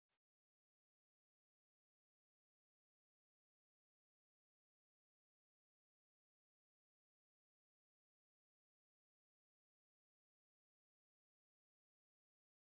PIV
Studio sperimentale dei vortici generati a valle di un profilo alare NACA 0012, con uso di un sistema PIV.

 Experimental PIV vortex analysis downstream of a NACA 0012 hydrofoil.